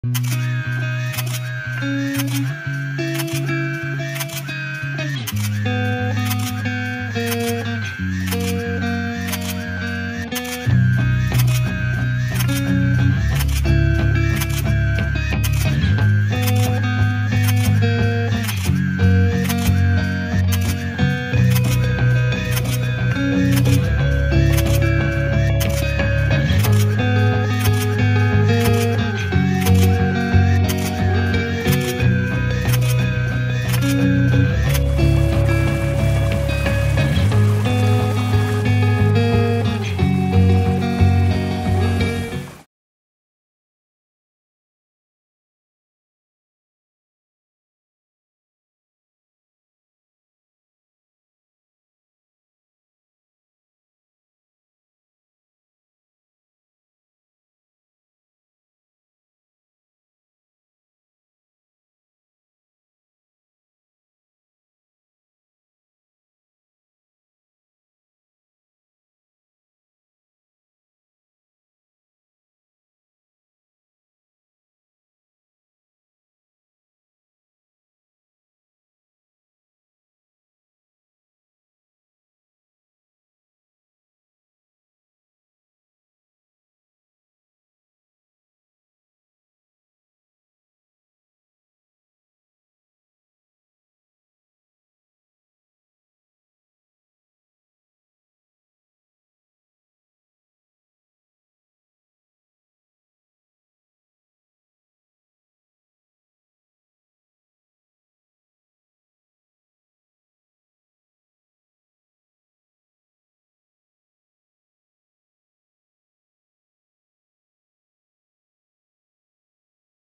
travels photographer
Viajes fotográficos en Cusco Perú, conoce lugares hermosos llenos de vida, Cusco nos ofrece variedad de paisajes y vida silvestre, se encuentra rodeado de lagunas, montañas y la entrada a la selva peruana, la puerta al Manu, viaja , conoce y consigue fotografías hermosas.